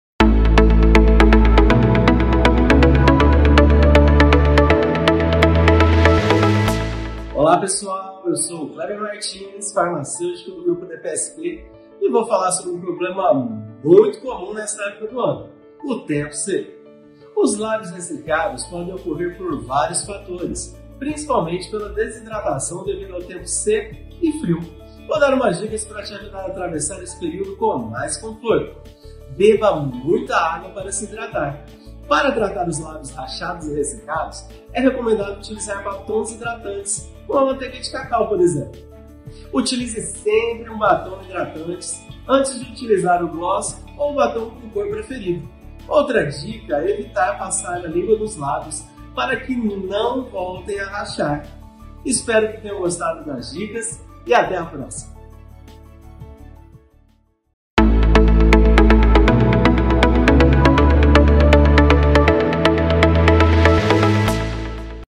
Drogaria São Paulo - Estação Seca
Mantenha os cuidados na Estação Seca.
Utilize hidratante labial para evitar a sensação de lábios rachados.